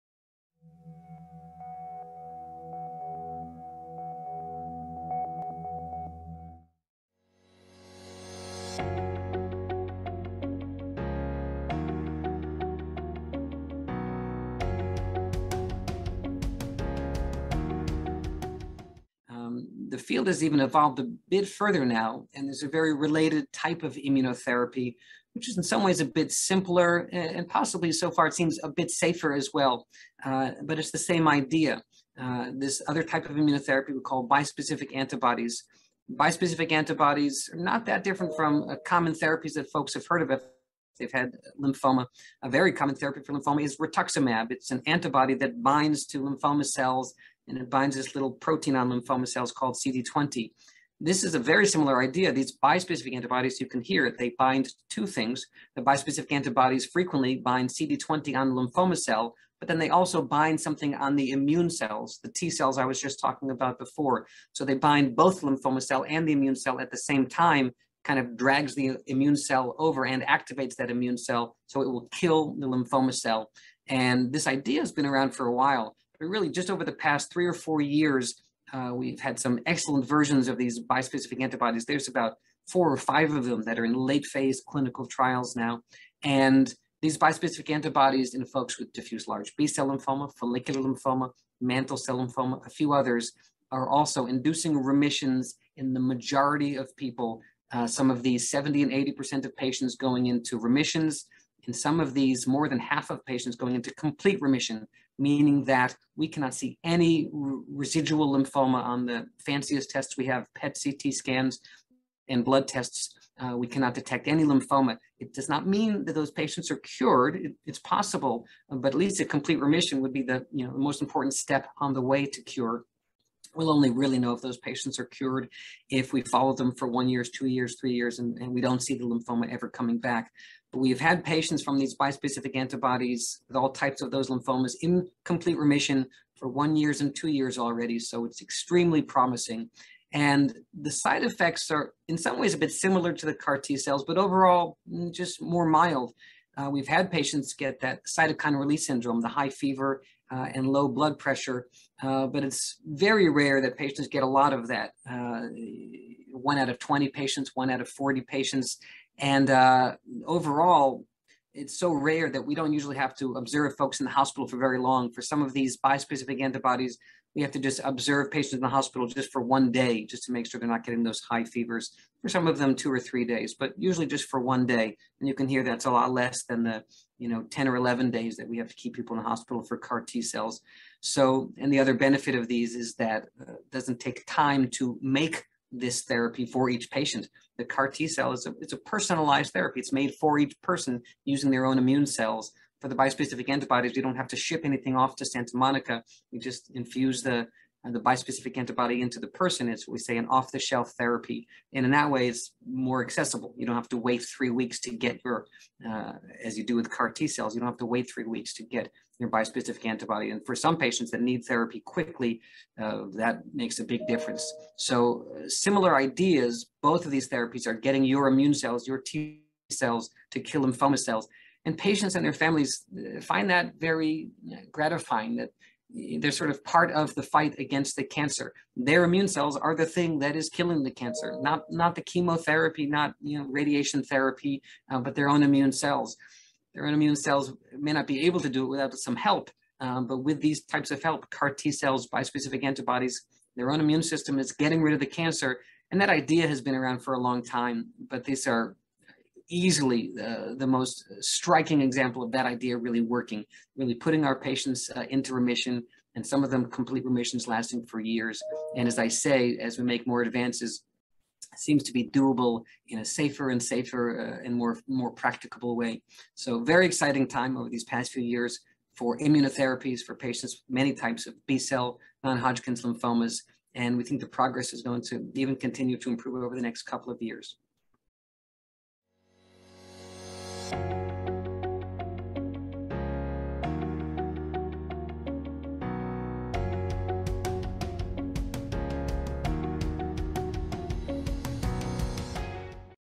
Immunotherapy Treatments B-Cell Non-Hodgkin's: Bispecific Antibodies - Blood Cancer Video Library
For the GRACE Blood Cancer Video Library, Dr. Joshua Brody, Associate Professor of Hematology and Medical Oncology, Associate Professor of Oncological Sciences and Director of the Lymphoma Immunotherapy Program at The Tisch Cancer Institute at Mount Sinai Hospital joins us to discuss emerging immunotherapy treatments for B-Cell Non-Hodgkin's Lymphoma: Bispecific Antibodies.

For this video update, Dr. Joshua Brody discusses the use of bispecific antibodies in the treatment of Non-Hodgkin's Lymphoma.